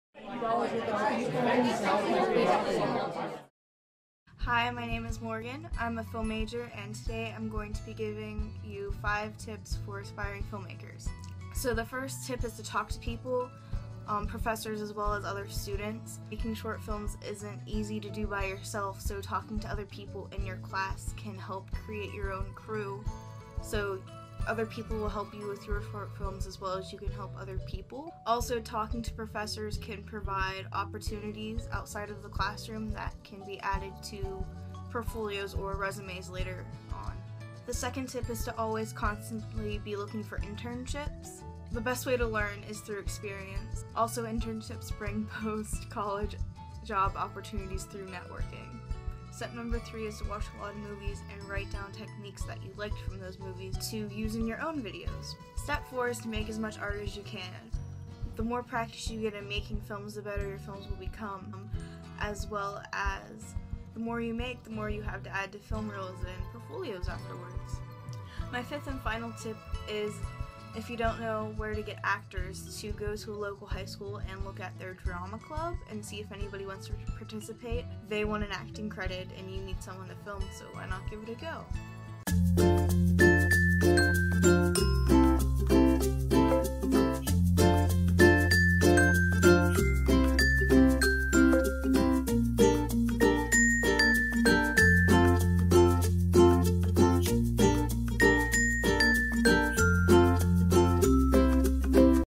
5 Tips for Aspiring Film Majors - College Conversations
If you're an aspiring film major, Morgan has 5 tips that will help you strengthen your resume and make your portfolio stand out from the crowd 🎥